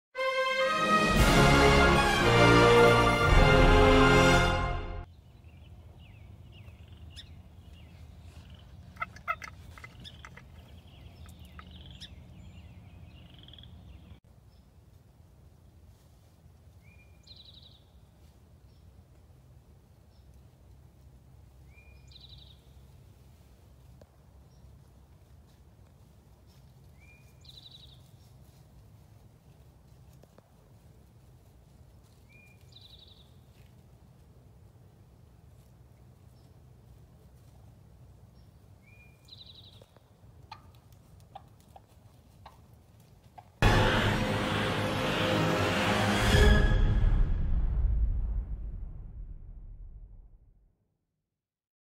Wild Turkey hunting May 2021 coyote, Turkey close encounters
Spring Wild Turkey hunting, the view from the Ameristep Out House ground blind, close encounter with a coyote and Turkey hen.